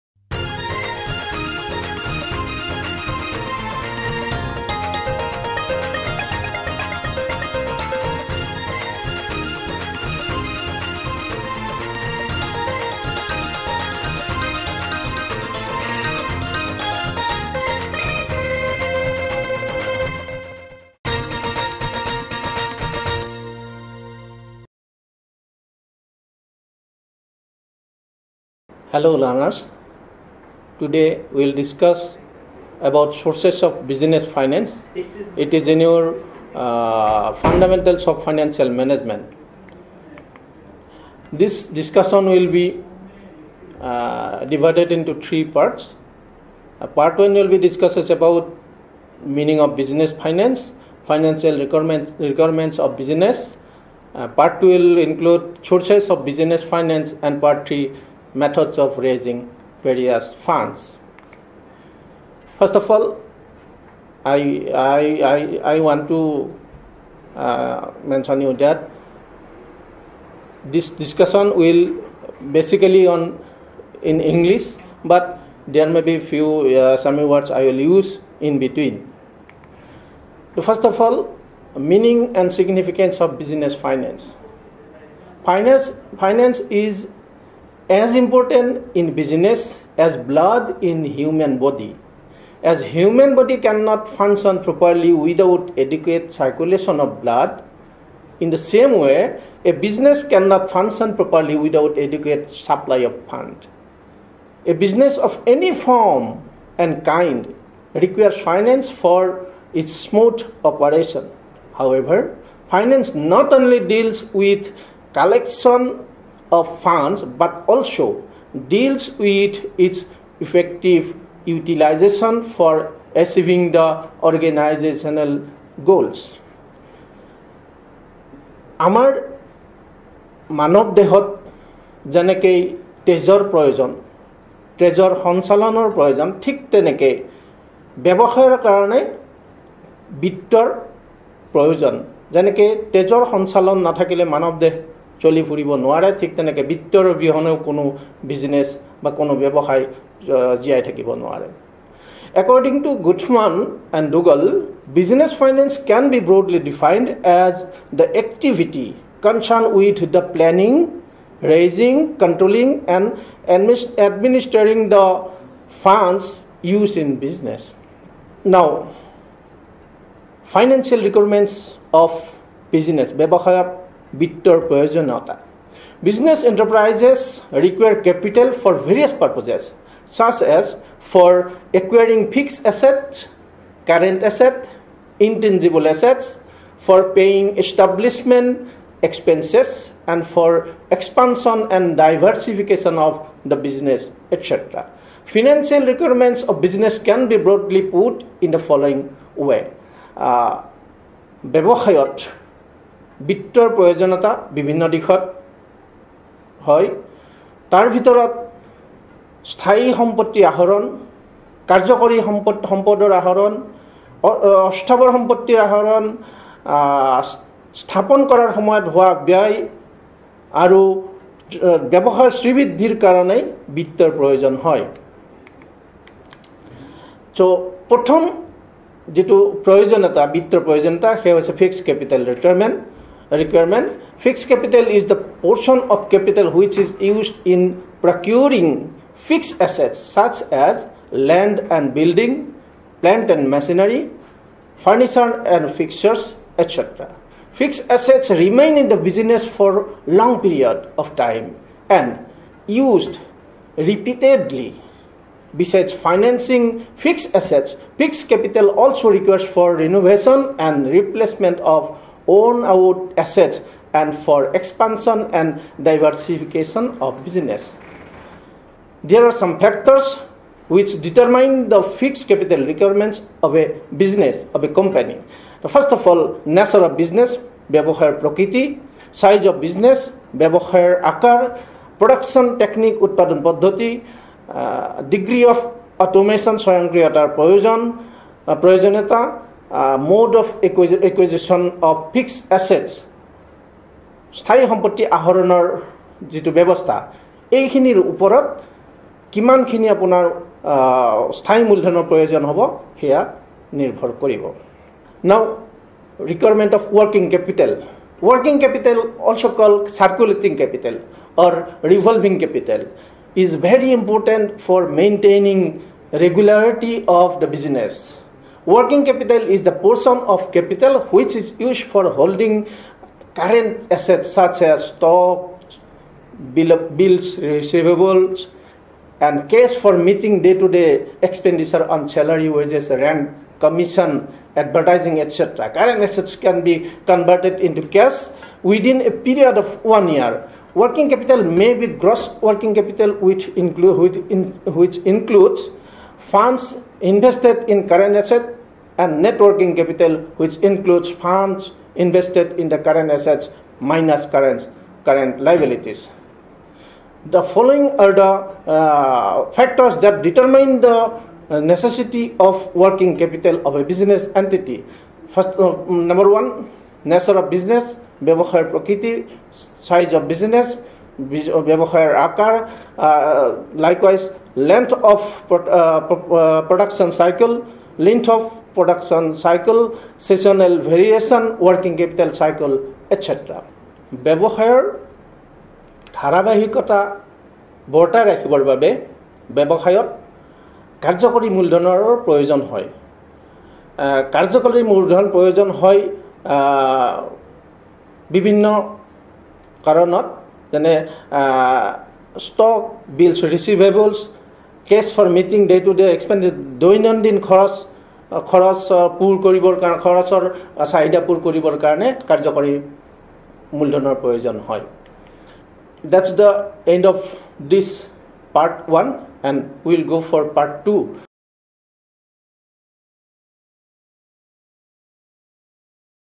B.Com.2nd Sem Course:FUNDAMENTALS OF FINANCIAL MANAGEMENT, UNIT-2,SOURCES OF BUSINESS FINANCE,Part-I
Presented by Dr. Gobinda Deka, Associate Professor, Maniram Dewan School of Management, KKHSOU. Edited by H Mout.